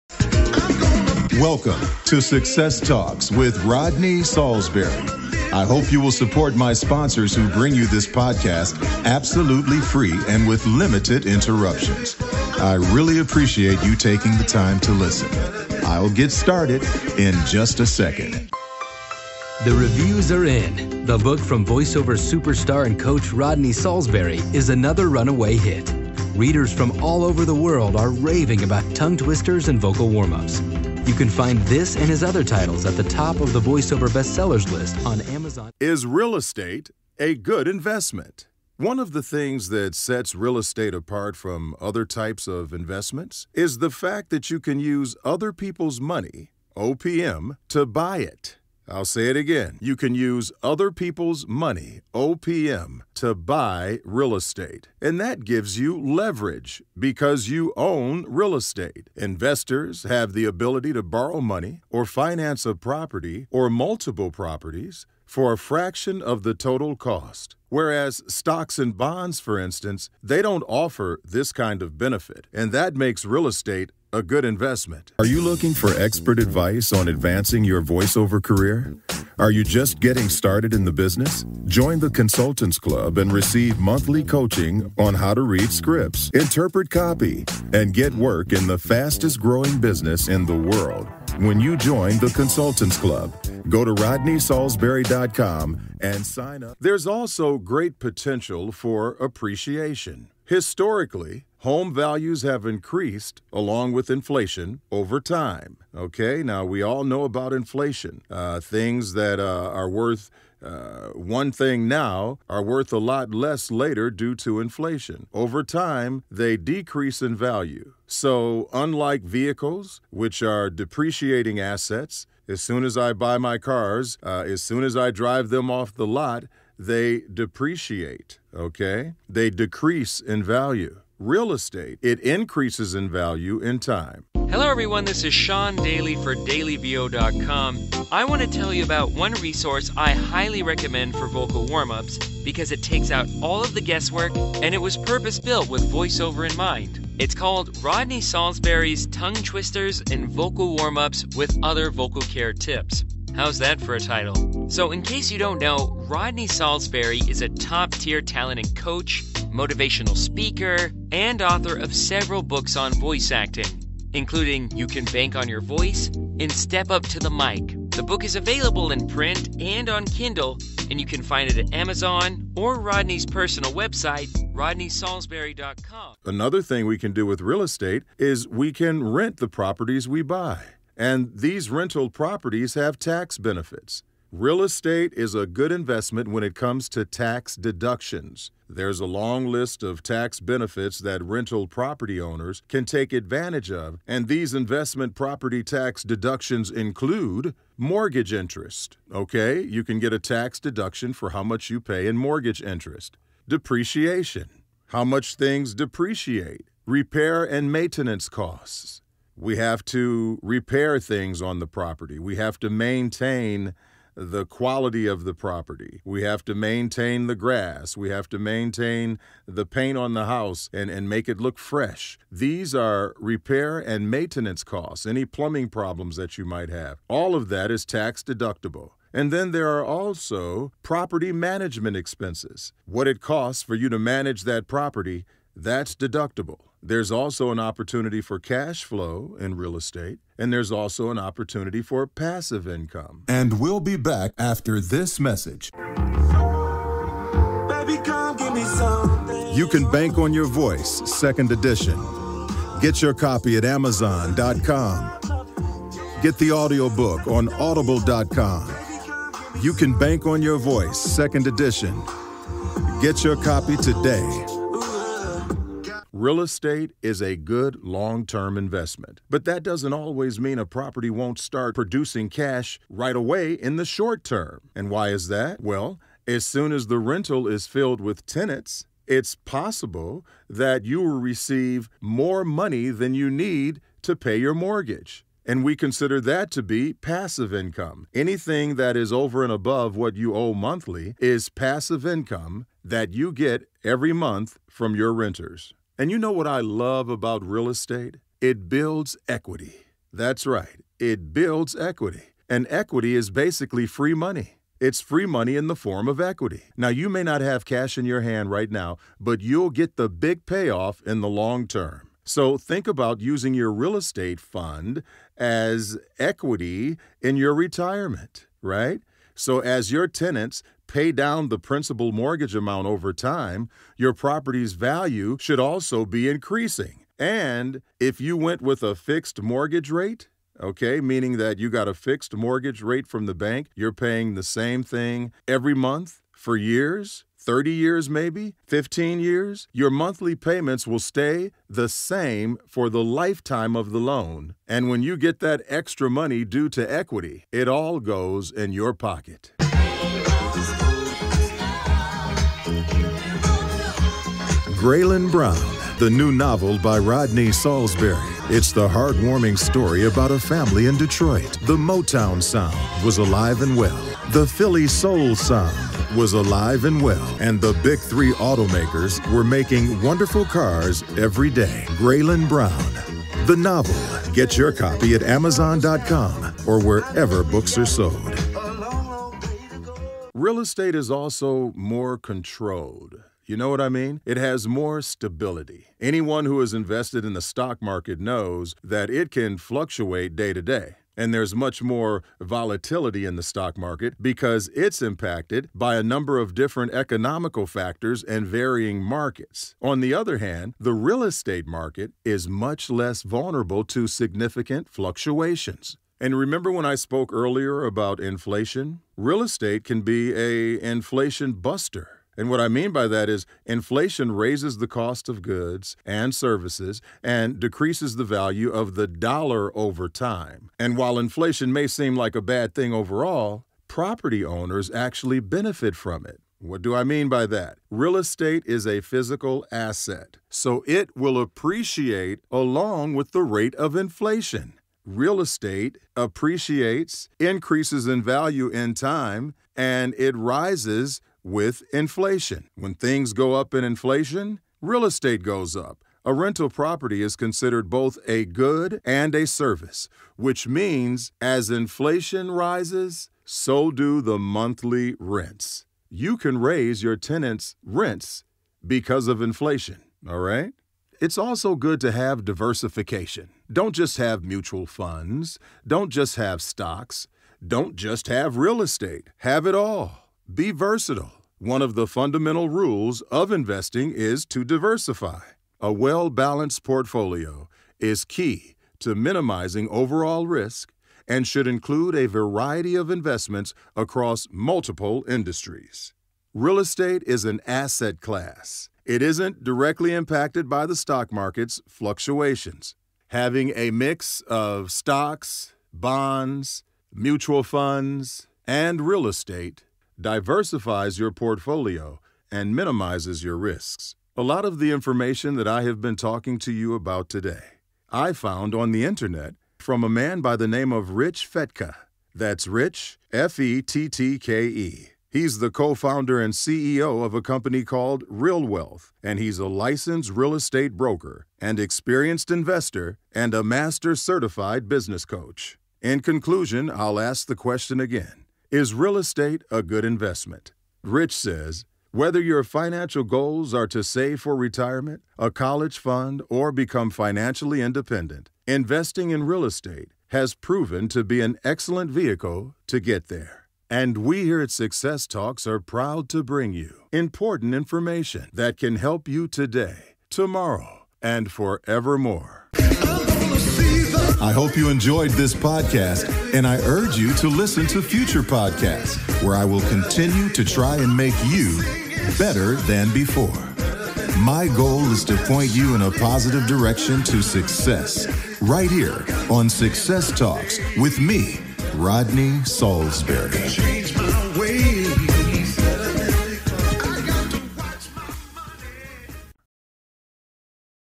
140 - Is Real Estate A Good Investment?
Voiceover Actor, Actor, Singer and Author Rodney Saulsberry narrates this discussion about real estate and its value as a solid investment. Saulsberry covers rental property, commercial real estate and diversifying your investment portfolio. He makes it plain as he expresses how real estate is inflation proof and constantly increases in value as inflation grows. He also makes mention of professionals who are certified investment coaches and advisors.